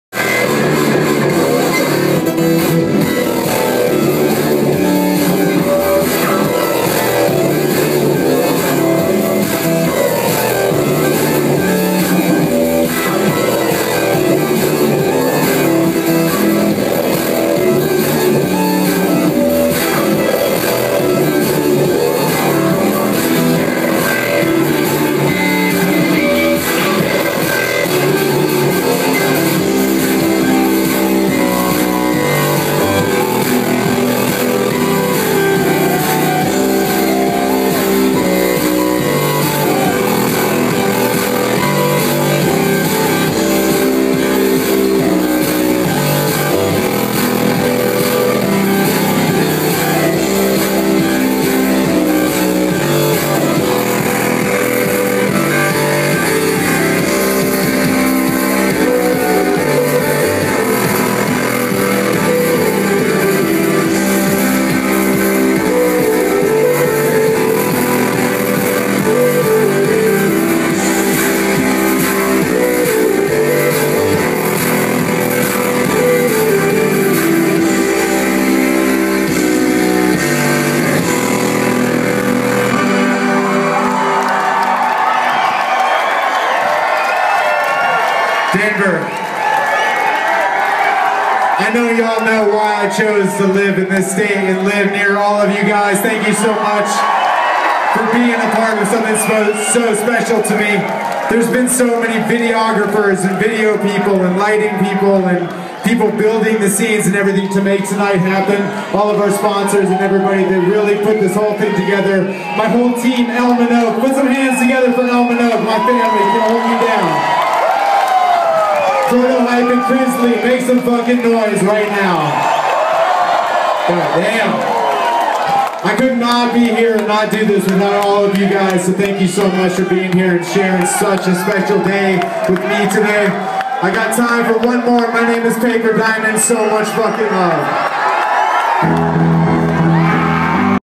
Paper Diamond Banger and Shout outs
Paper Diamond rocks it hard then gives a shout out to all those who support him and Colorado in General.